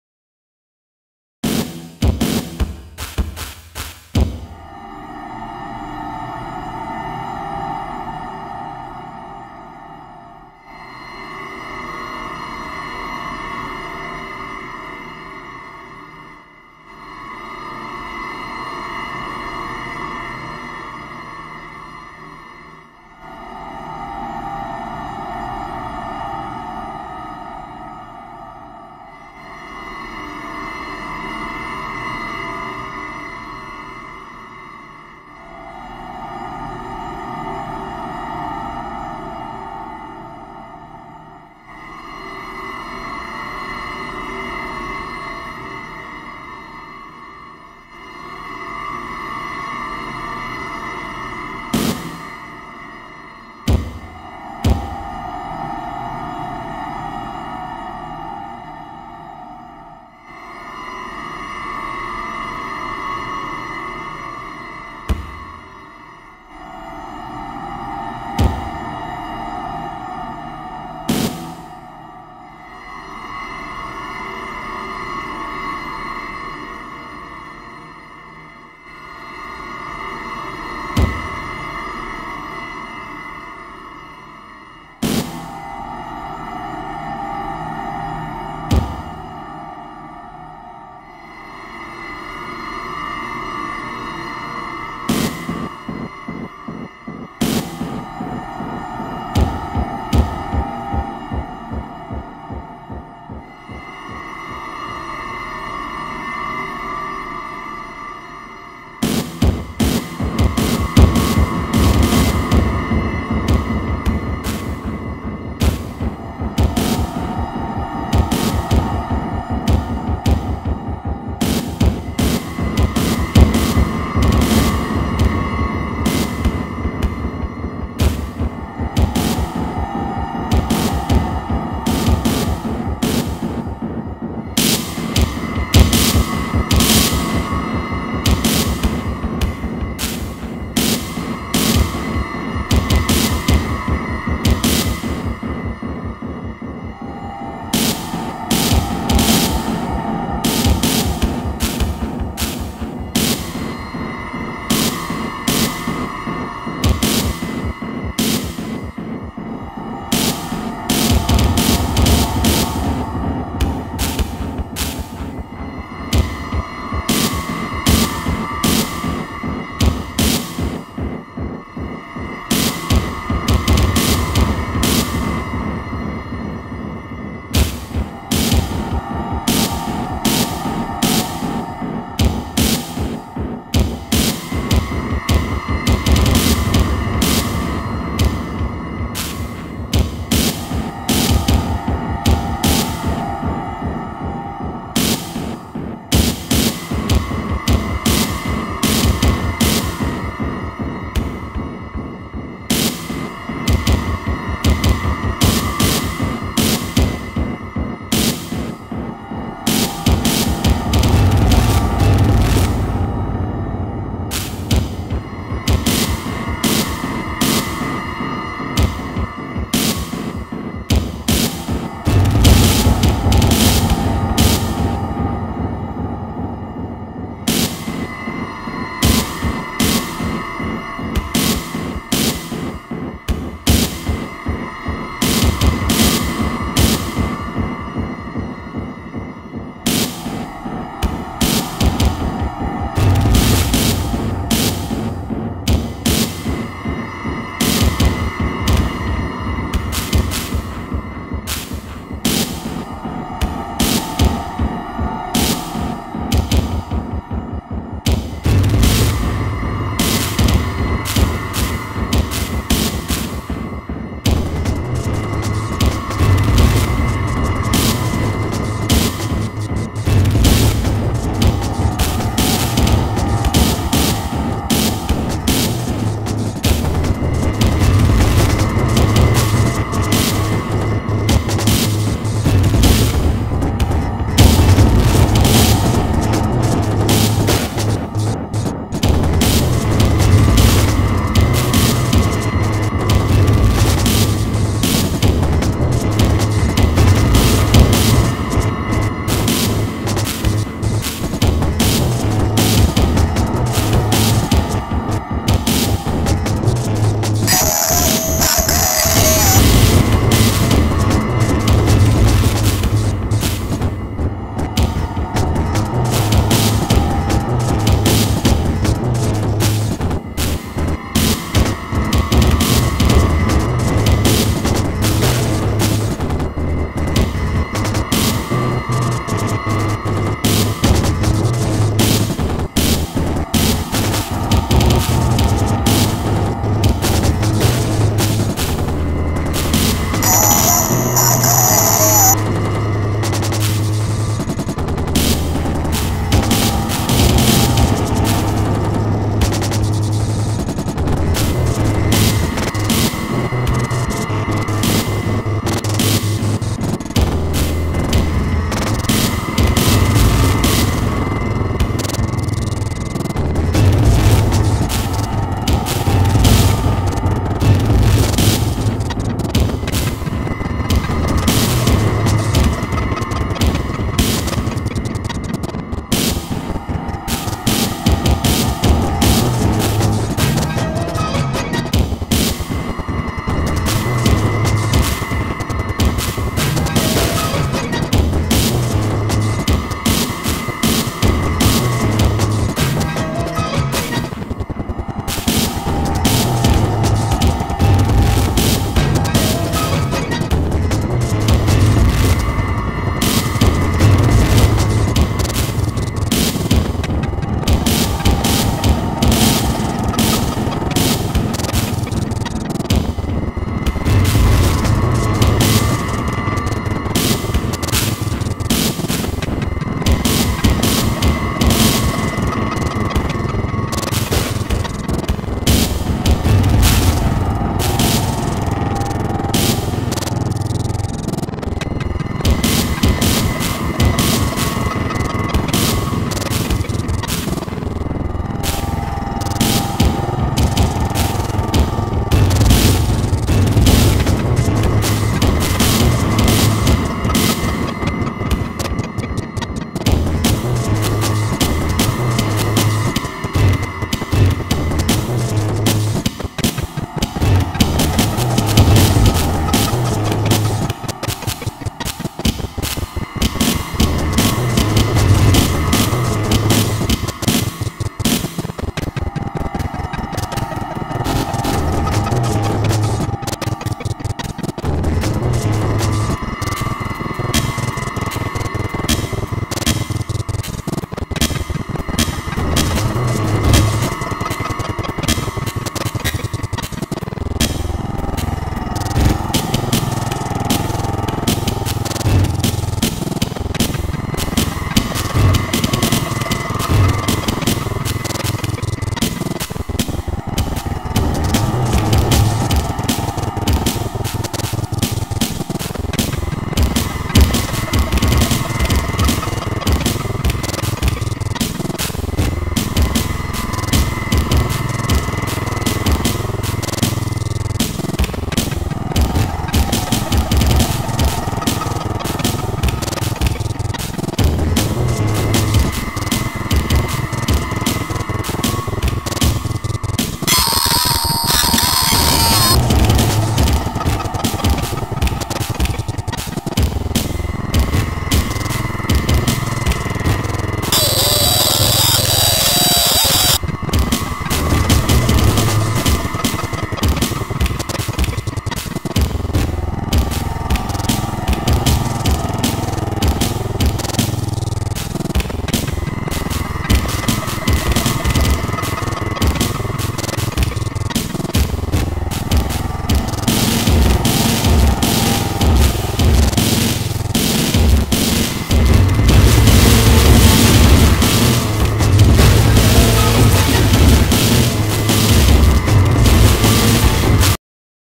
Algorave 10th Birthday March 2022 - bgold - 2022-03-20 03:00
Algorave 10th Birthday March 2022 live performance by bgold from SSP, MN, USA

Tidal